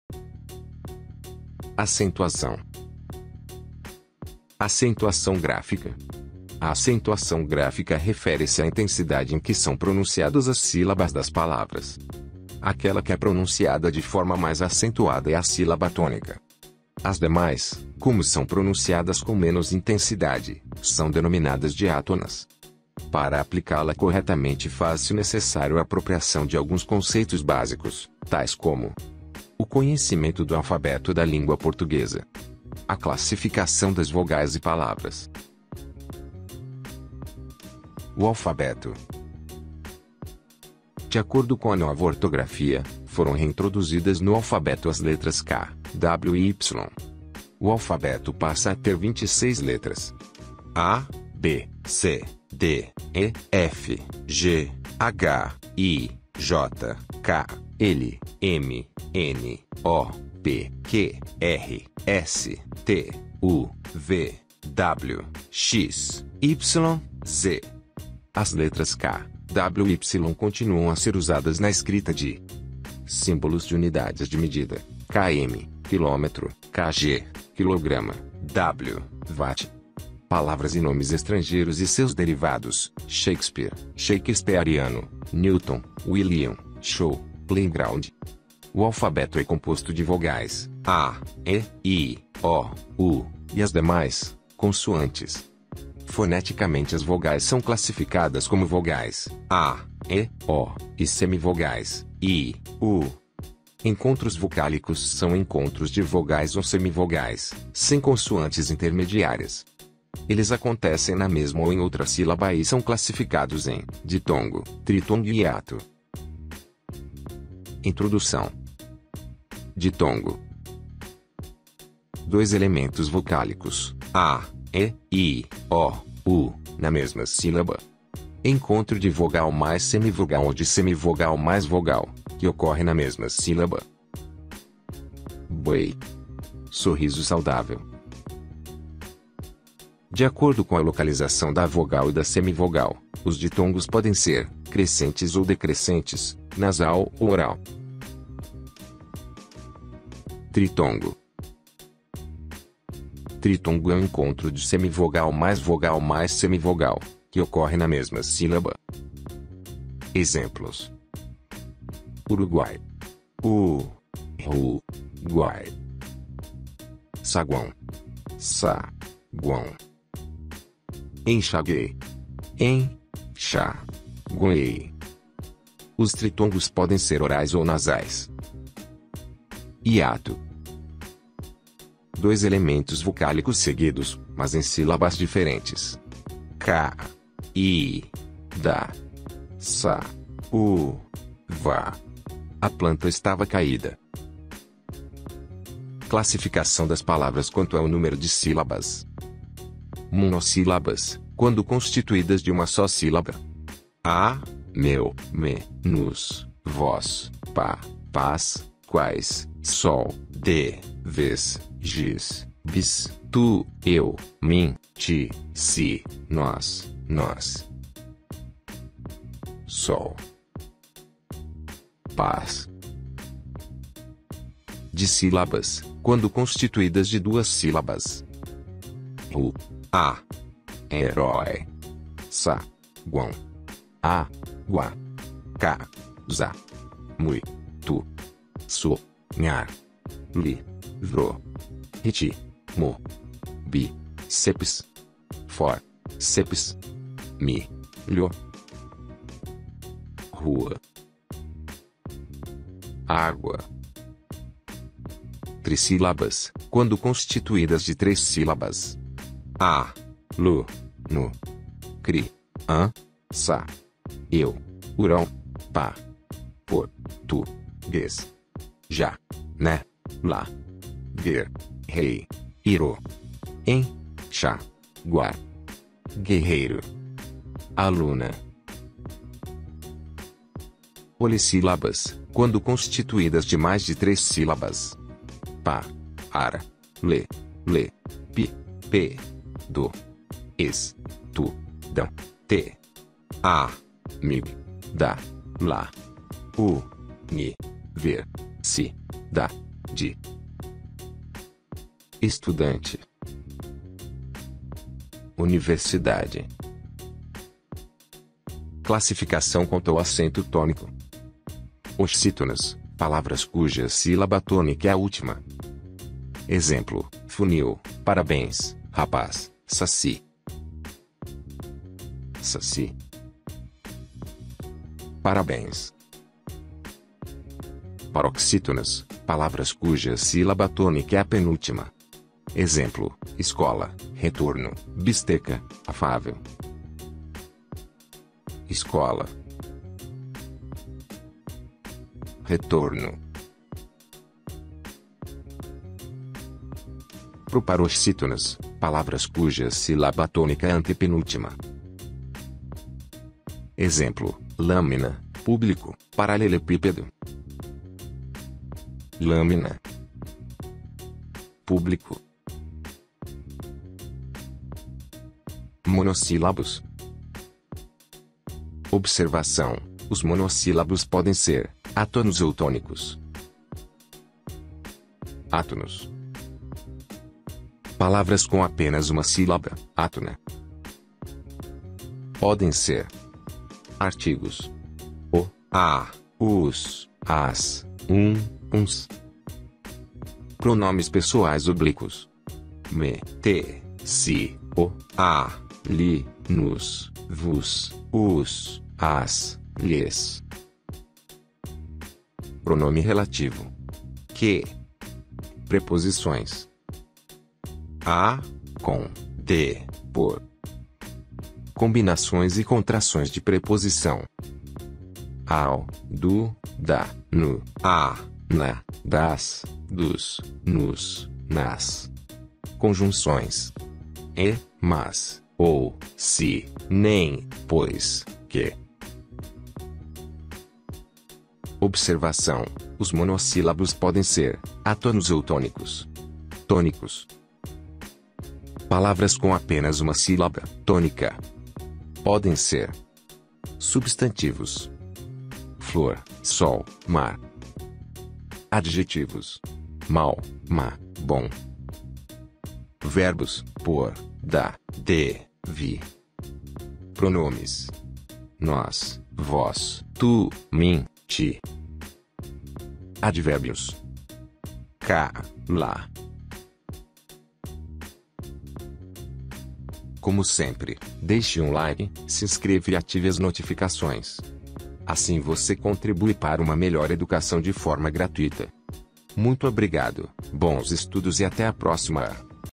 Acentuação Gráfica - Regras de acentuação
Introdução ao conteúdo acentuação. Reveja conteúdos básico para a apropriação e compreensão das regras de acentuação!